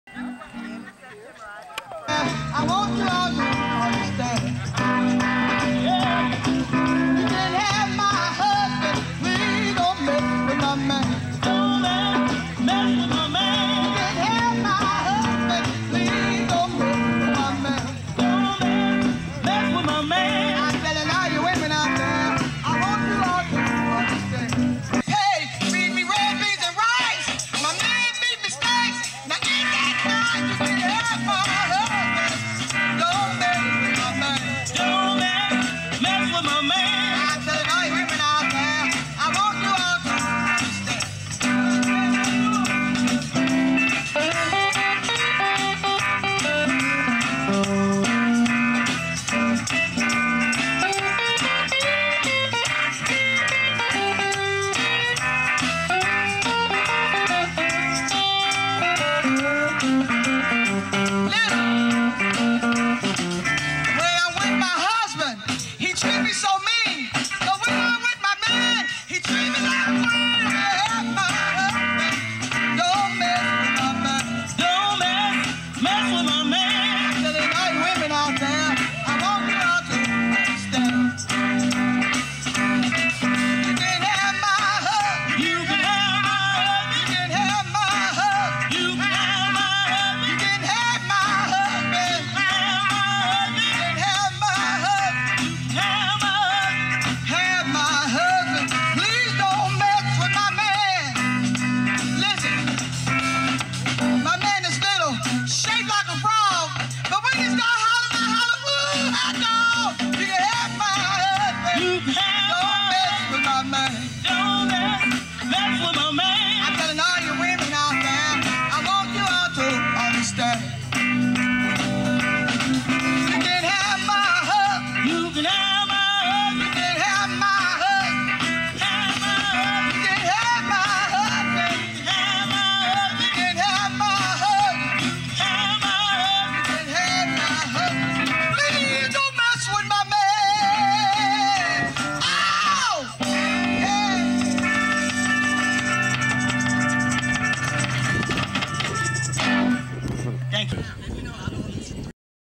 Smith's Creek Blues Festival_summer2002
The 'Blues Creators' @ the 'Smith's Creek Blues Festival' in St. Clair County, Michigan.